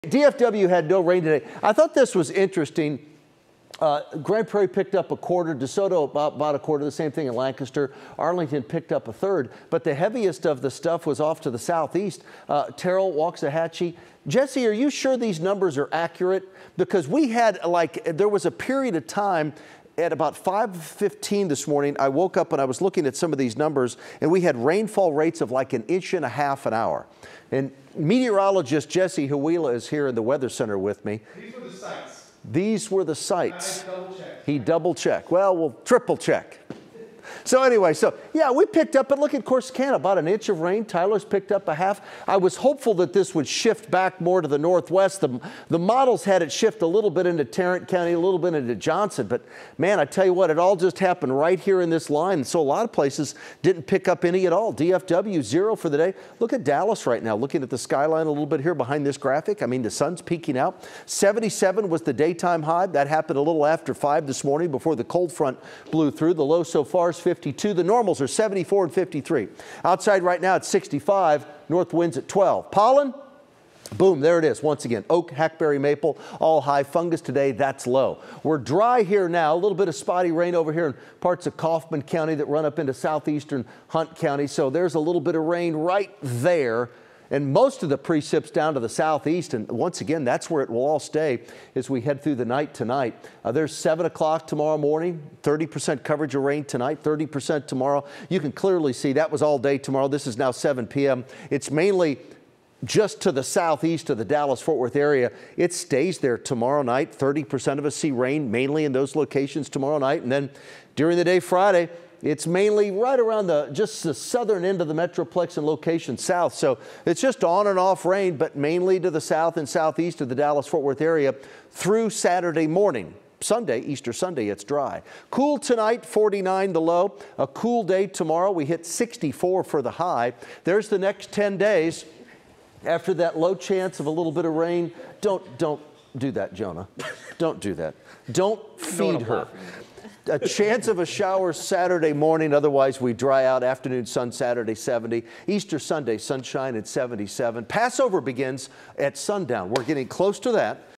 DFW weather: A look at the next rain chances on Thursday
Another chance of rain is in the forecast on Thursday.